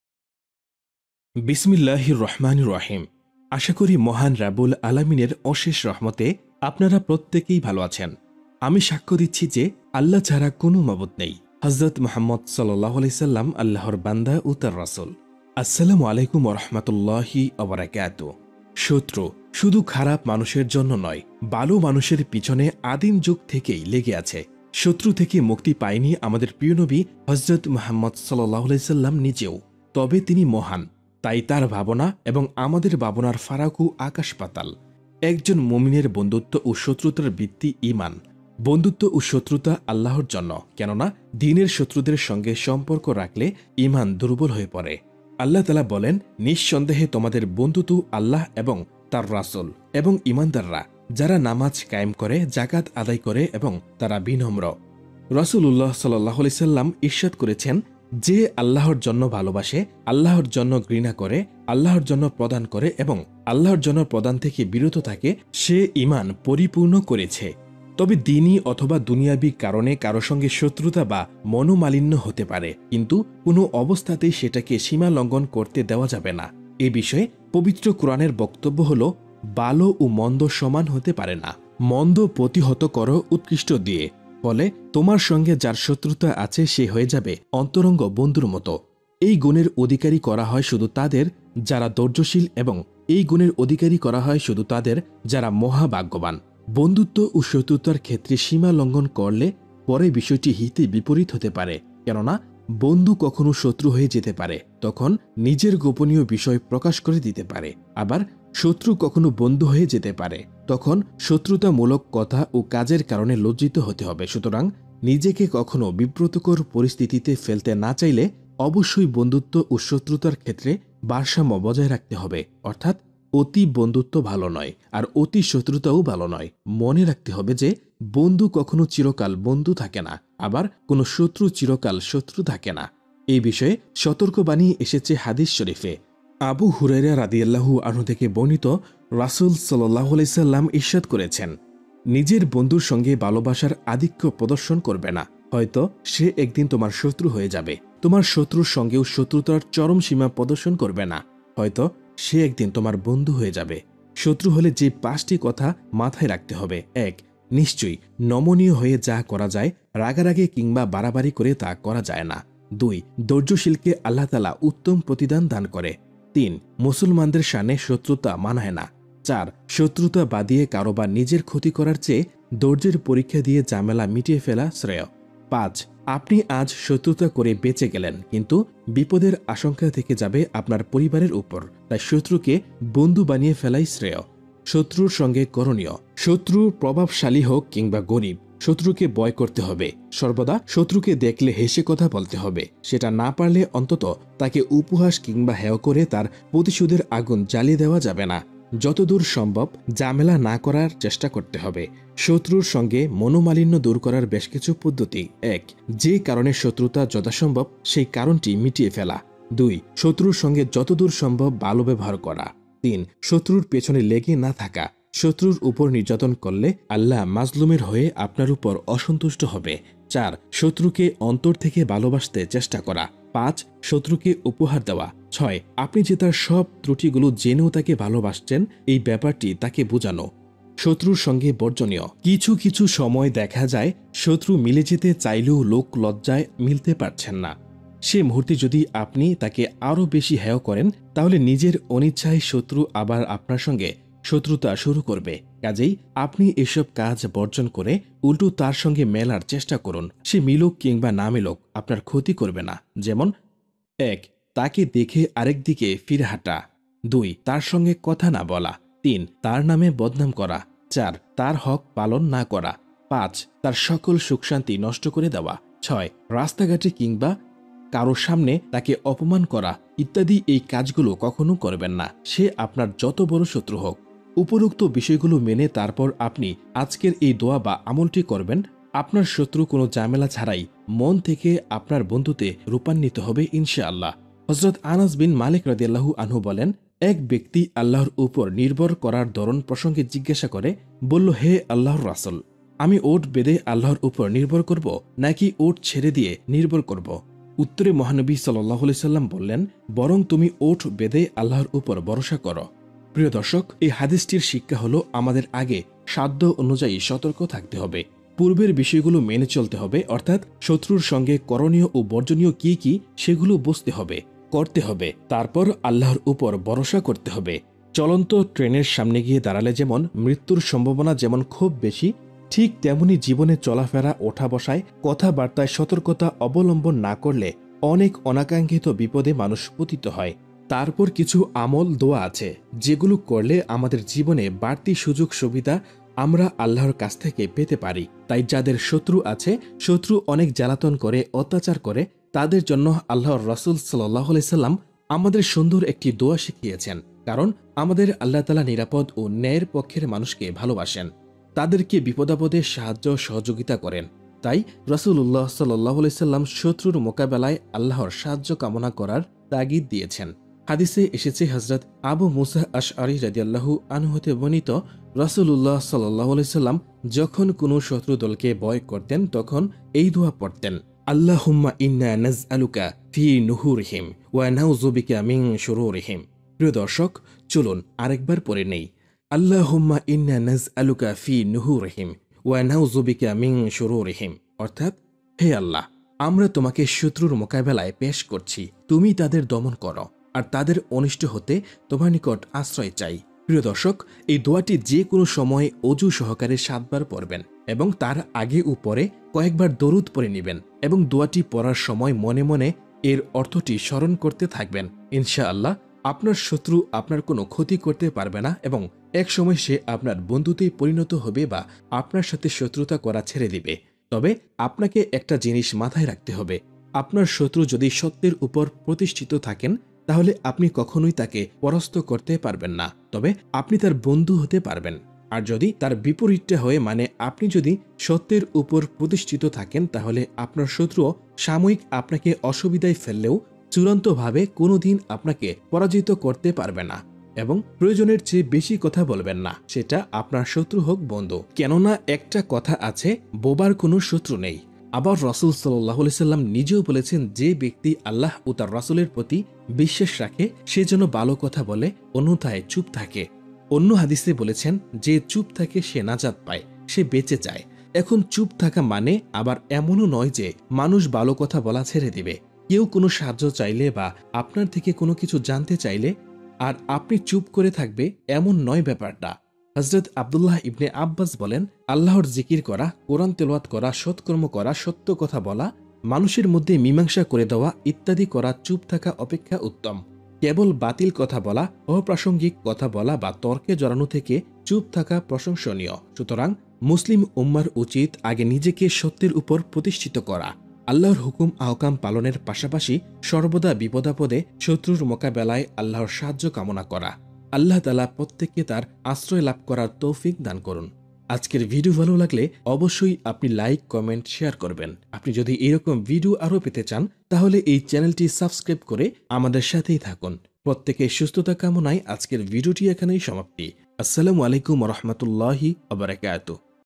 শত্রু যদি খুব বেশি জ্বালায়, অত্যাচার করে দোয়াটি ৭বার পড়ুন
শত্রু শুধু খারাপ মানুষের জন্যে নয়, ভালো মানুষের পিছনে আদিম যুগ থেকেই লেগে আছে। শত্রু থেকে মুক্তি পায়নি আমাদের প্রিয় নবী হজরত মুহাম্মদ (সা.) নিজেও।  তবে তিনি মহান।  তাই তার ভাবনা এবং আমাদের ভাবনার ফারাকও আকাশ পাতাল। একজন মুমিনের বন্ধুত্ব ও শত্রুতার ভিত্তি ঈমান। বন্ধুত্ব ও শত্রুতা আল্লাহর জন্য। কেননা দ্বিনের শত্রুদের সঙ্গে সম্পর্ক রাখলে ঈমান দুর্বল হয়ে পড়ে।

বন্ধুত্ব ও শত্রুতার ক্ষেত্রে সীমা লঙ্ঘন করলে পরে বিষয়টি হিতেবিপরীত হতে পারে। কেননা বন্ধু কখনো শত্রু হয়ে যেতে পারে। তখন নিজের গোপনীয় বিষয় প্রকাশ করে দিতে পারে। আবার শত্রু কখনো বন্ধু হয়ে যেতে পারে। তখন শত্রুতামূলক কথা ও কাজের কারণে লজ্জিত হতে হবে। সুতরাং নিজেকে কখনো বিব্রতকর পরিস্থিতিতে ফেলতে না চাইলে অবশ্যই বন্ধুত্ব ও শত্রুতার ক্ষেত্রে ভারসাম্য বজায় রাখতে হবে। অর্থাৎ অতি বন্ধুত্ব ভালো নয়, আর অতি শত্রুতাও ভালো নয়। মনে রাখতে হবে যে বন্ধু কখনো চিরকাল বন্ধু থাকে না, আবার কোনো শত্রু চিরকাল শত্রু থাকে না।

শত্রু হলে যে ৫টি কথা মাথায় রাখতে হবে-
১. নিশ্চয়, নমনীয় হয়ে যা করা যায়, রাগারাগি কিংবা বাড়াবাড়ি করে তা করা যায় না।
২.  ধৈর্যশীলকে আল্লাহ তায়ালা উত্তম প্রতিদান দান করে।
৩. মুসলমানদের শানে শত্রুটা মানায় না।
৪. শত্রুতা বাধিয়ে কারও বা নিজের খতি করার চেয়ে ধৈর্যের পরীক্ষা দিয়ে ঝামেলা মিটিয়ে ফেলা শ্রেয়।
৫. আপনি আজ শত্রুতা করে বেচেঁ গেলেন,
কিন্তু বিপদের আশংকা থেকে যাবে আপনার পরিবারের উপর। তাই শত্রুকে বন্ধু বানিয়ে ফেলাই শ্রেয়।